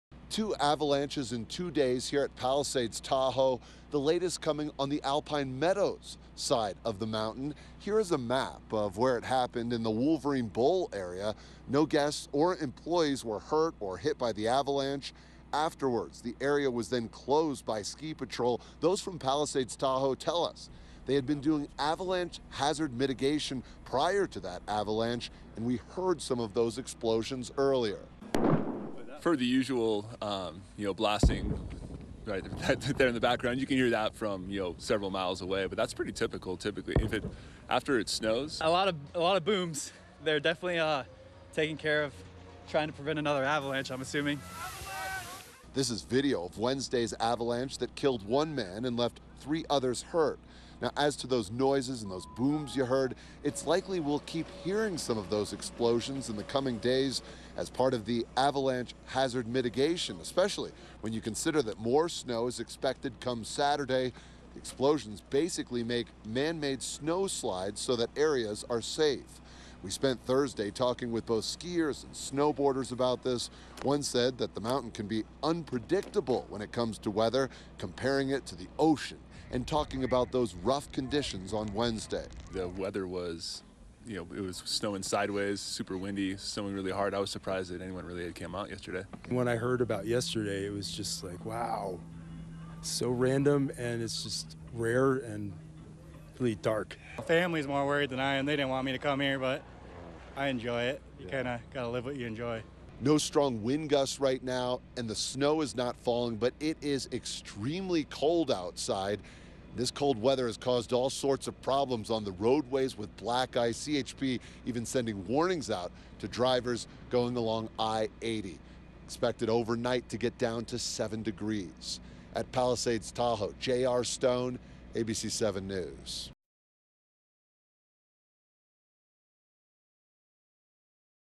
2nd avalanche strikes CA ski resort, day after different event kills man
Palisades Tahoe ski resort says a second avalanche struck the mountain on Thursday, one day after a different one killed a Bay Area man and injured three others.